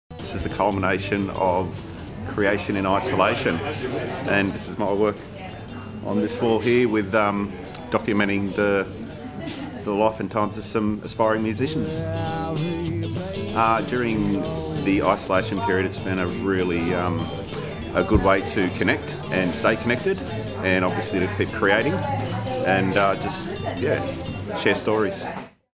Neighbourhoods Art Trail 2021 - Creation In Isolation exhibition with Matt Briggs.
Neighbourhoods was a community-led art initiative facilitated by Central Coast Council. A project aimed at having conversations and exploring art within and around the Central Coast community. Part of Neighbourhoods was the Art Trail which took place on the 19th of June, 2021 in Long Jetty, NSW. This video captures the "creation in isolation" work of photographer Matt Briggs at the Grant Molony Gallery, Long Jetty.

Video created by Treehouse Media